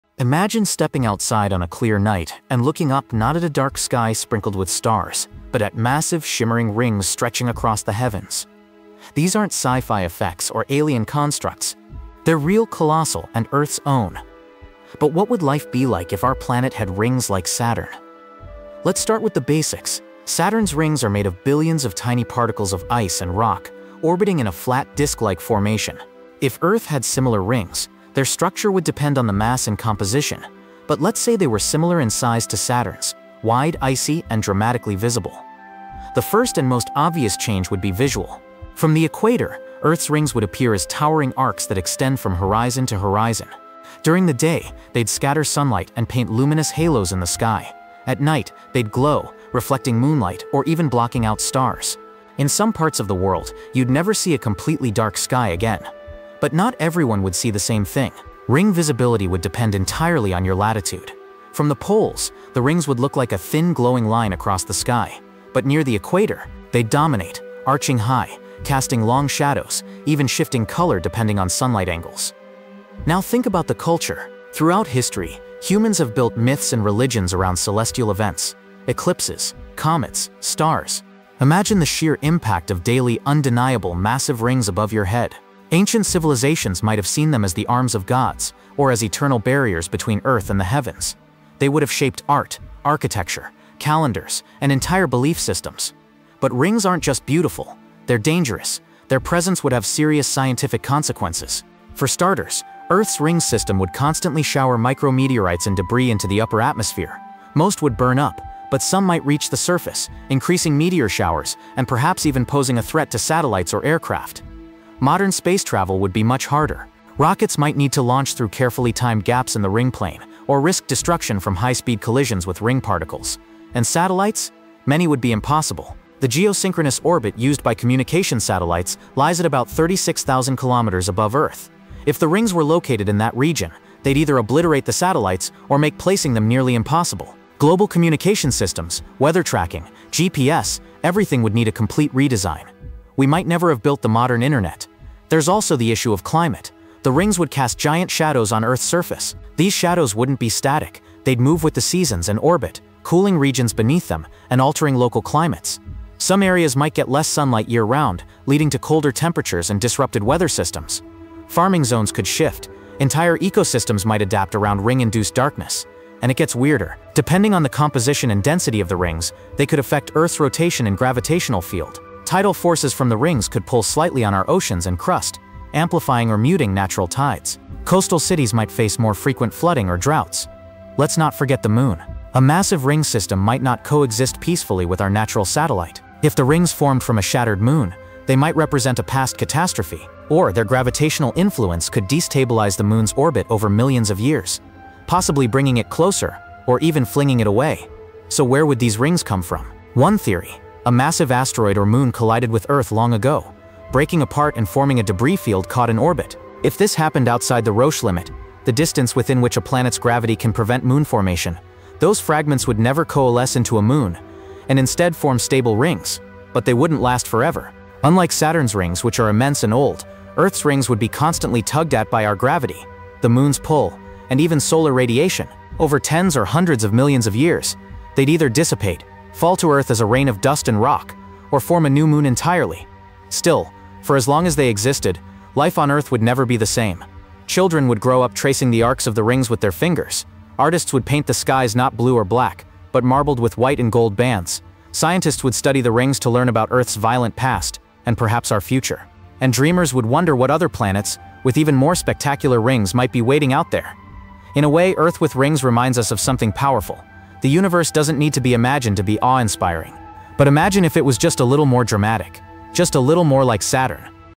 What If Earth Had Rings Like Saturn?
Imagine looking up and seeing colossal glowing rings stretch across the sky, visible even in daylight. What if Earth had rings like Saturn? How would our tides, weather, culture, and even history change under their shadow? In this video, we explore the stunning beauty—and surprising dangers—of an Earth wrapped in cosmic rings.